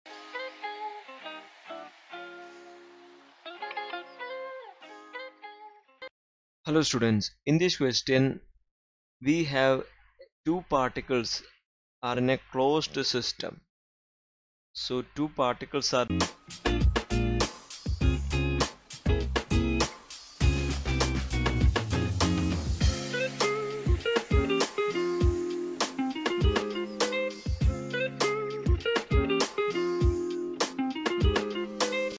If these two particles are a closed system, what do you know about the total energy as their separa…
If these two particles are a closed system, what do you know about the total energy as their separation changes? b) In general, what is the minimum value of KE that a system of particles can have? c) Use your response to the two previous parts of this question and the fact that the total energy is equal to -0.8e to determine the two values of r where KE is minimum. d) Explain how your responses to all of the above determine the range of possible values of the separation of the two particles for this total energy.